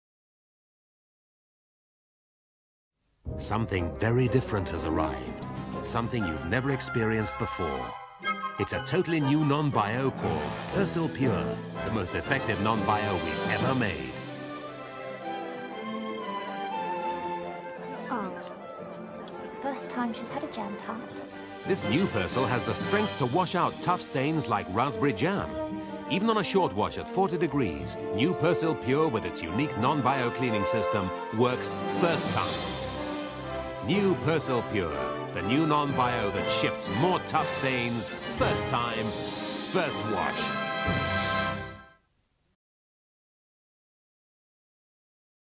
1994: Persil' Pure Non-Bio [First Time] - 40sec Advert
Title: First Time
Brand: Persil
Advertiser/Client: Lever
Agency: -
Media: TV Advert
Featuring: -
Guest Appearance: -
Voice Over: -
Year: 1994
Location: -
Country: UK
Washing Machine: Hoover New Wave
Model: -
Slogan: Persil Pure Non-Bio.
Facts: Persil Power came to the UK market during the early 90s. Persil produced a detergent so powerful that it damaged your clothing.

(Repost: Originally uploaded on The Laundry Lab, June 2011)